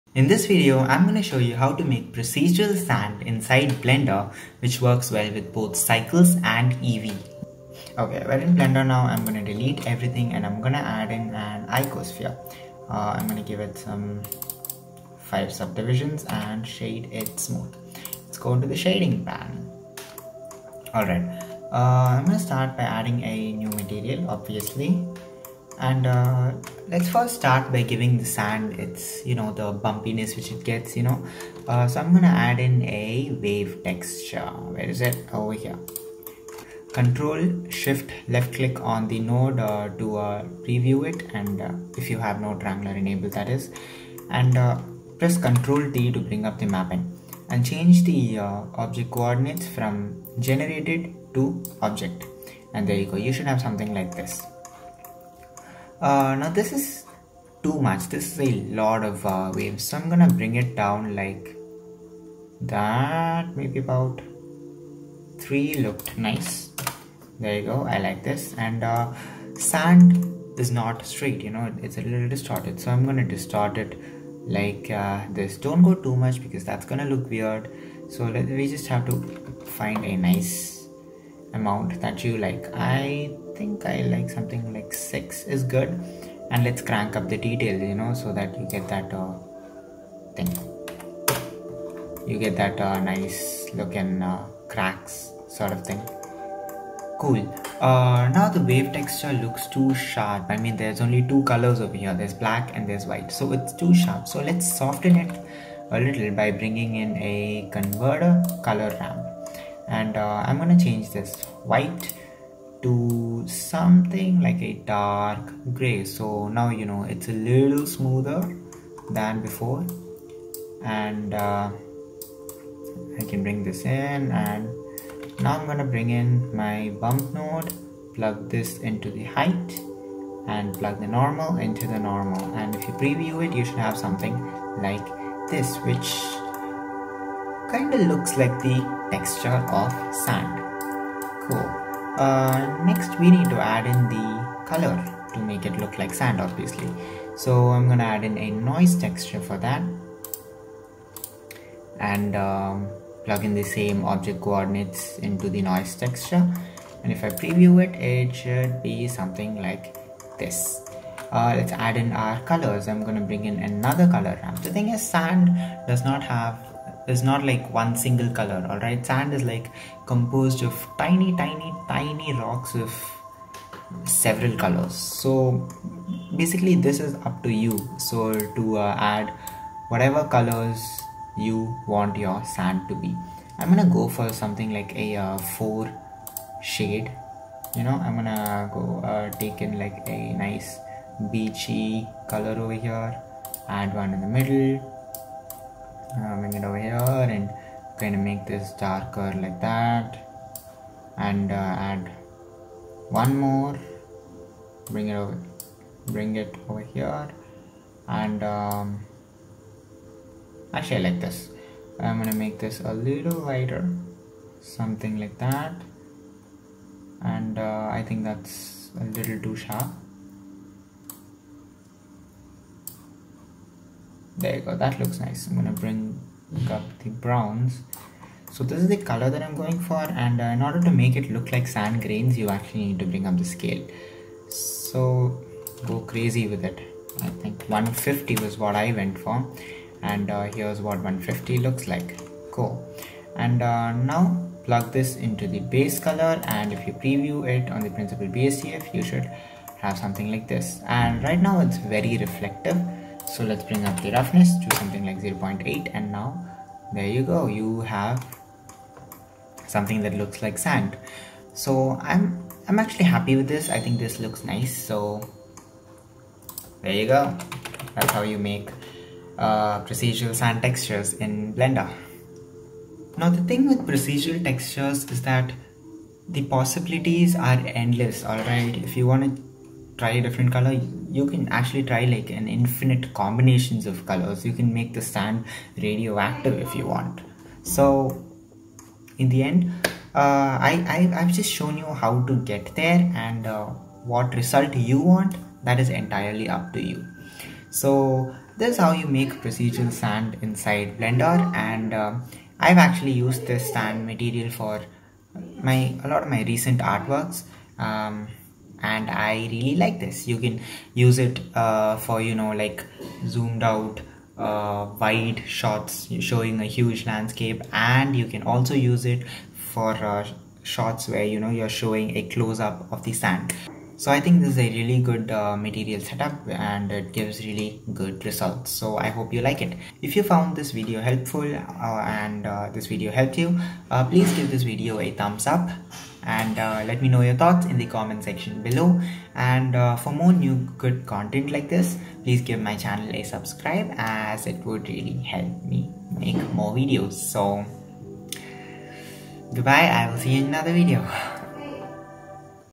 Quick and easy procedural sand material for cycles and eevee (Blender tutorial)
Hi!
In this video, let me show you a simple node setup to make a procedural sand material in Blender, that works with both cycles and eevee.